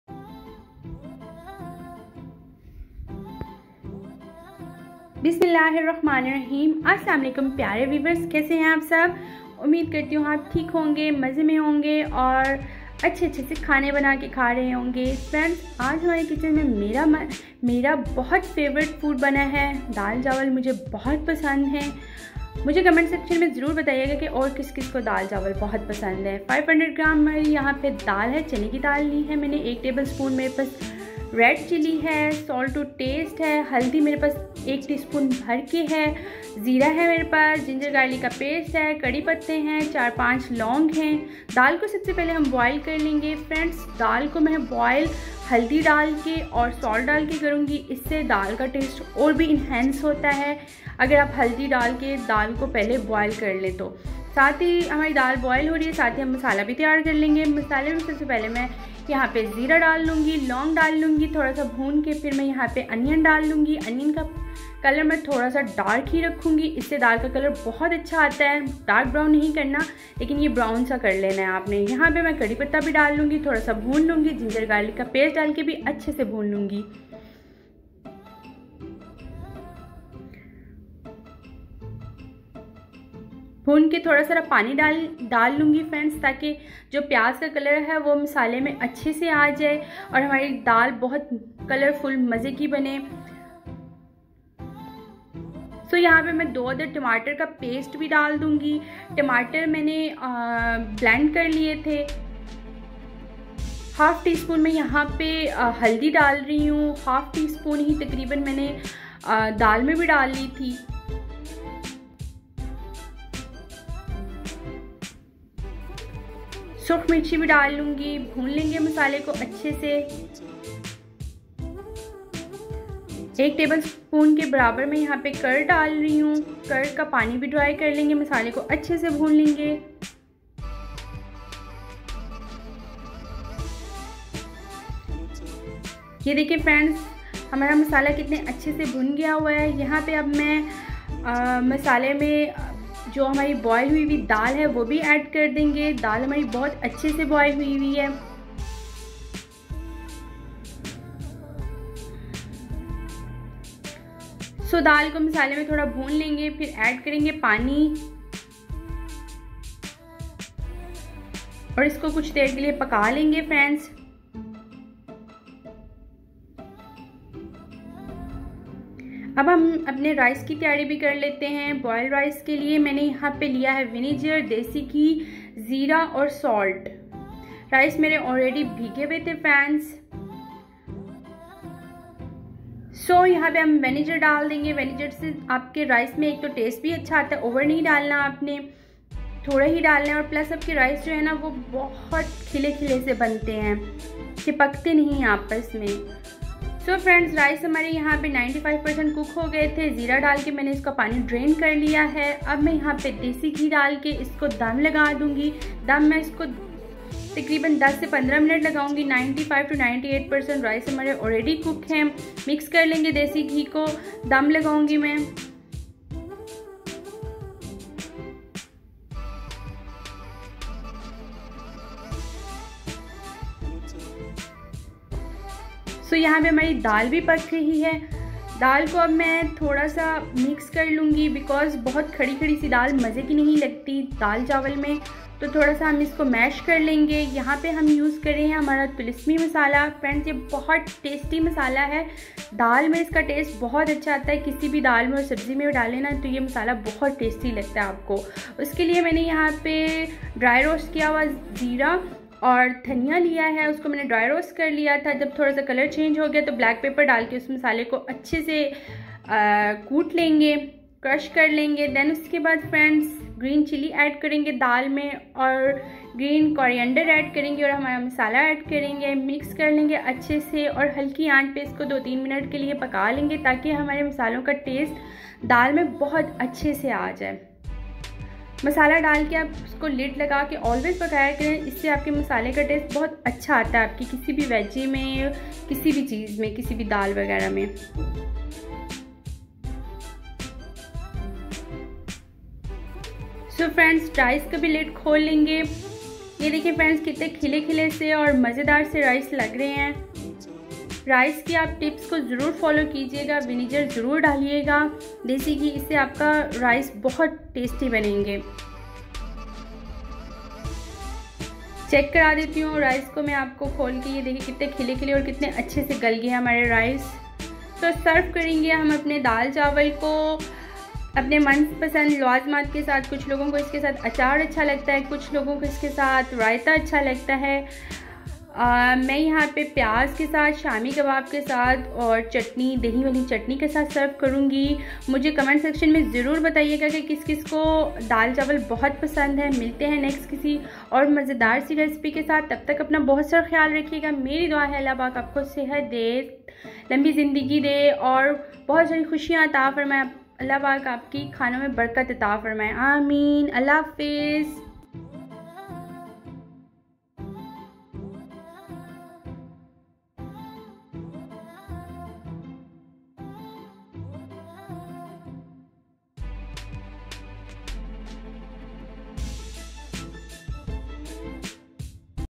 Dal Chawal Recipe | How To Make Dal Rice | Traditional Veg Recipe
Daal Chawal is a popular dish that is largely eaten in Asian countries like India, Pakistan, and Nepal.  It consists of lentils and rice. The lentils are cooked until mushy, typically flavored with cumin and coriander for extra flavor. Jeera Rice is an Indian-style rice dish flavored with cumin.

Dal Rice
Daal Chawal
Chana Dal Rice
Chana Daal Chawal

Recipe By Life With Sehr